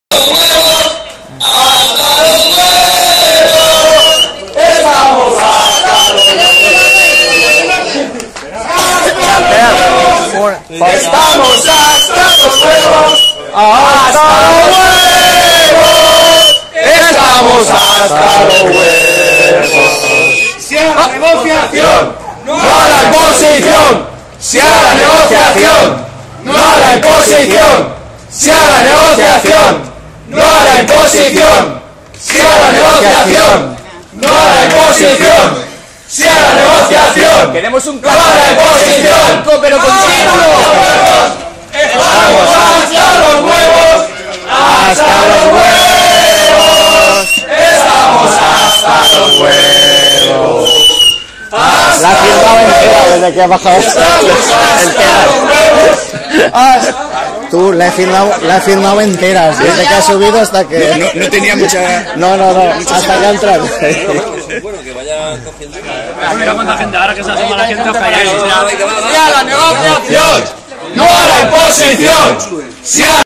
Manifestación de la Policía Local de Benicasim (2012)
Manifestación de miembros de la Policía Local de Benicasim. En el vídeo se aprecia como aparece la concejala del Partido Popular Carolina San Miguel